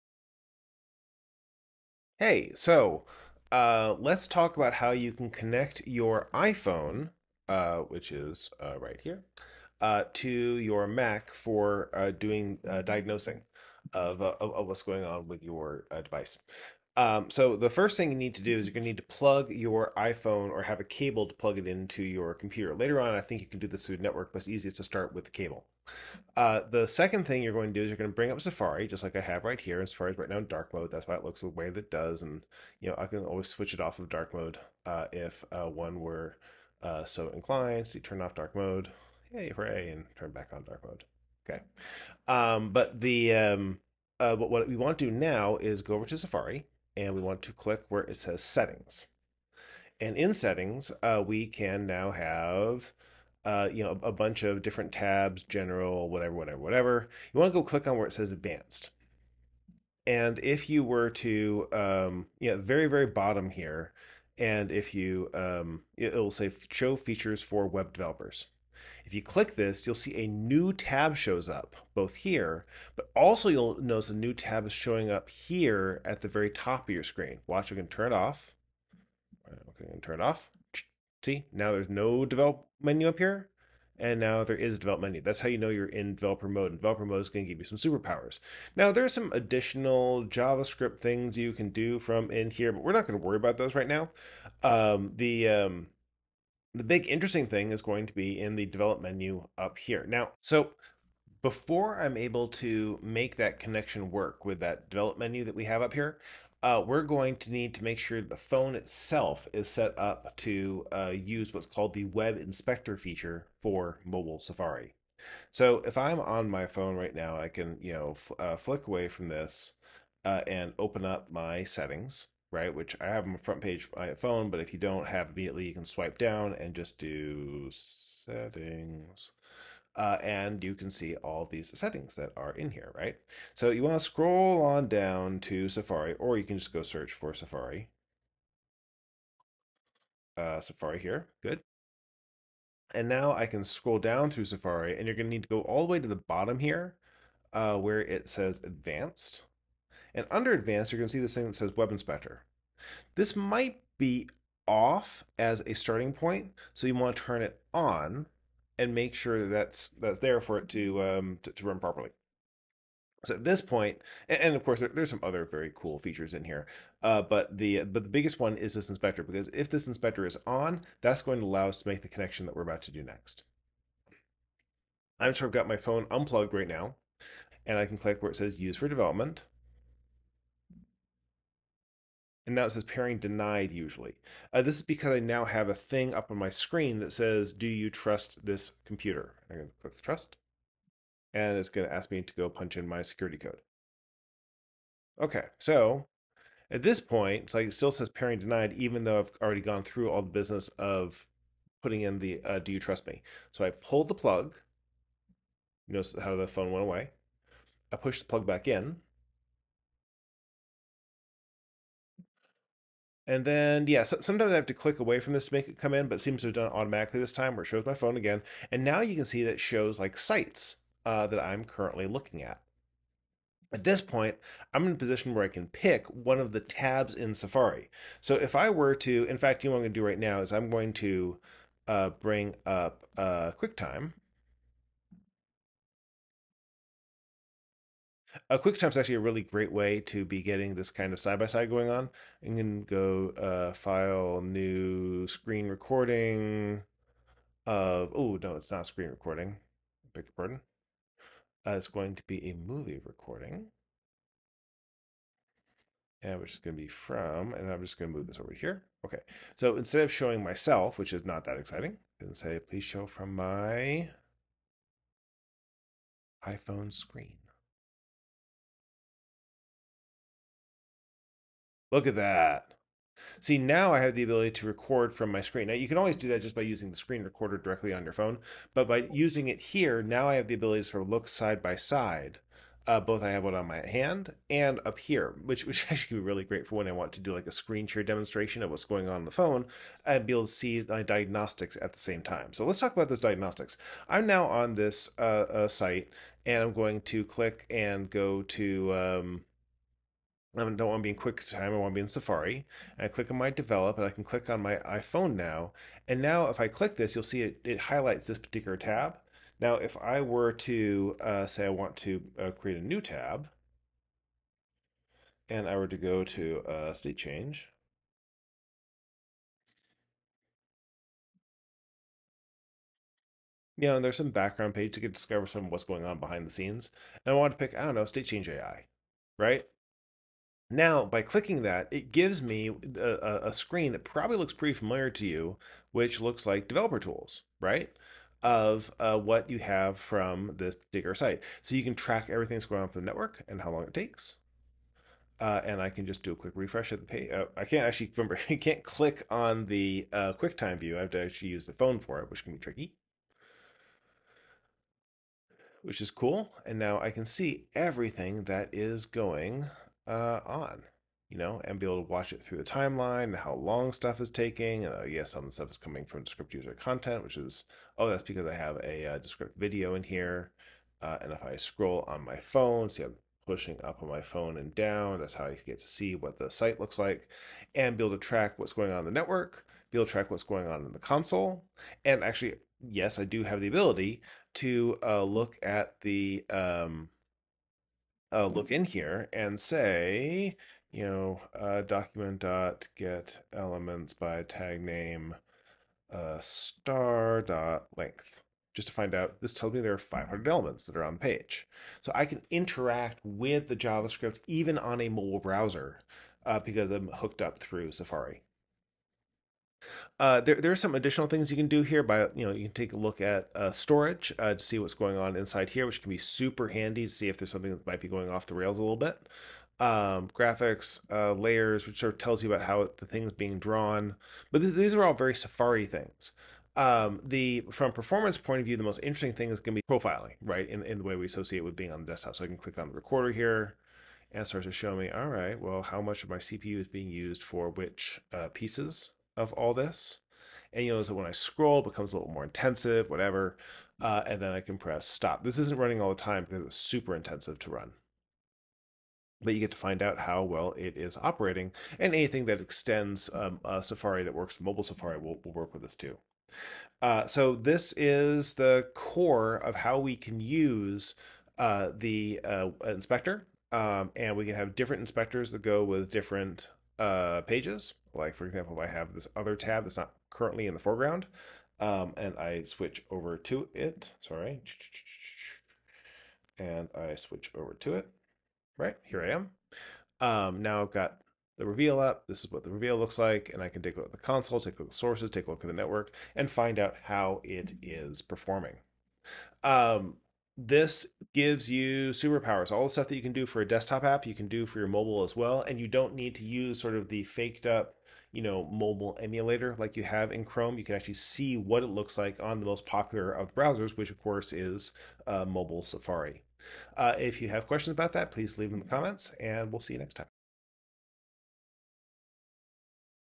Diagnose and Fix Your iPhone Web Apps with Web Inspector
This is a video about Diagnose and Fix Your iPhone Web Apps with Web Inspector

0:00 Diagnose and Fix Your iPhone Web Apps with Web Inspector
00:41 Turn On Developer Settings in Safari
01:40 Setting Up Your Phone for Web Inspection
02:54 Pairing the Phone with your Desktop
04:52 Tour of Web Inspector Tools
07:07 Profiler for Deep Investigation